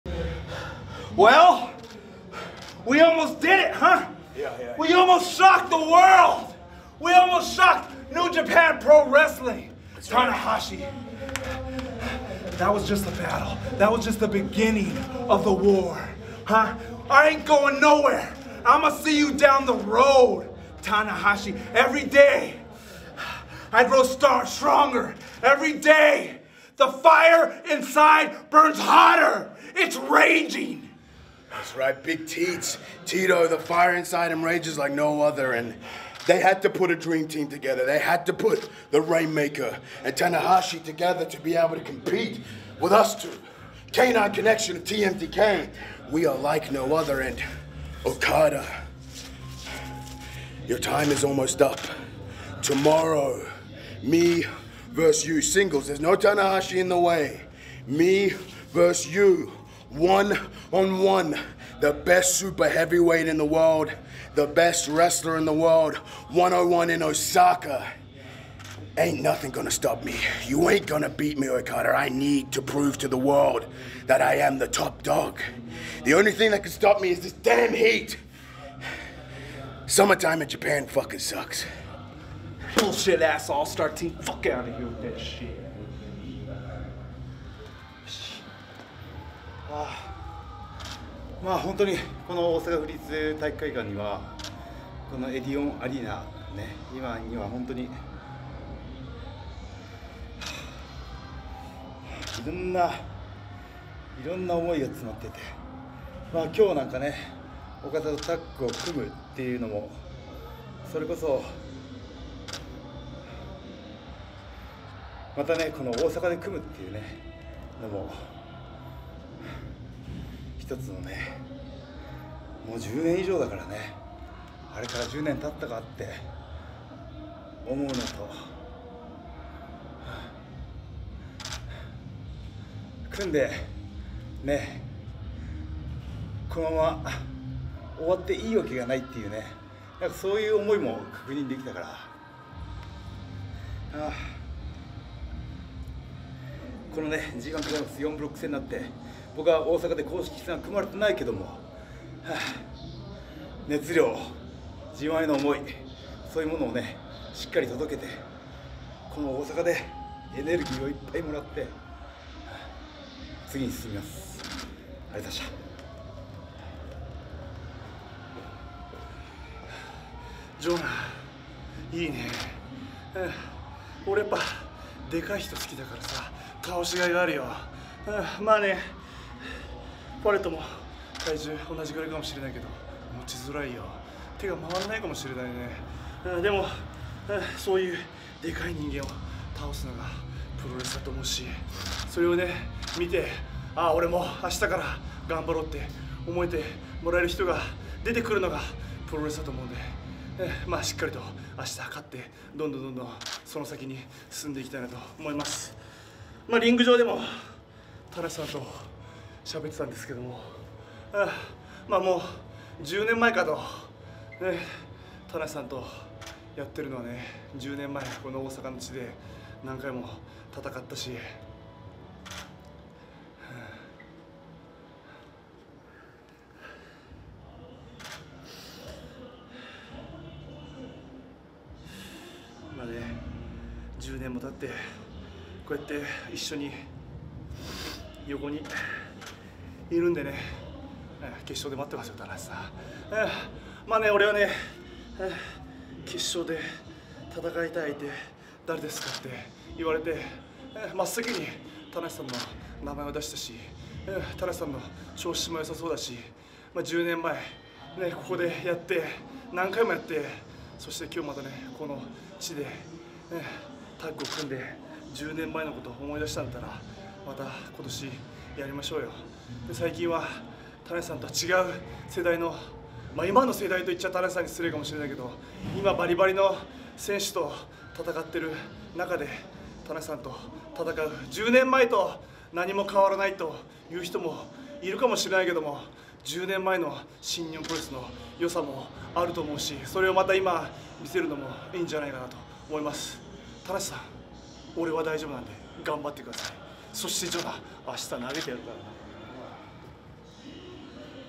オカダ「10年も経って、こうやって一緒に、横にいるんでね。決勝で待ってますよ、棚橋さん」8.6 G1CLIMAX32 Backstage comments: 4th match
2022年8月6日(土)G1 CLIMAX 32
大阪・大阪府立体育会館（エディオンアリーナ大阪）
第4試合：
棚橋 弘至 & オカダ・カズチカ 🆚 バッド・デュード・ティト & ジョナ

August 6, 2022 “G1 CLIMAX 32 Day12”
Osaka・OSAKA PREFECTURAL GYM (EDION ARENA OSAKA)
4th match:
Hiroshi Tanahashi and Kazuchika Okada 🆚 Bad Dude Tito and JONAH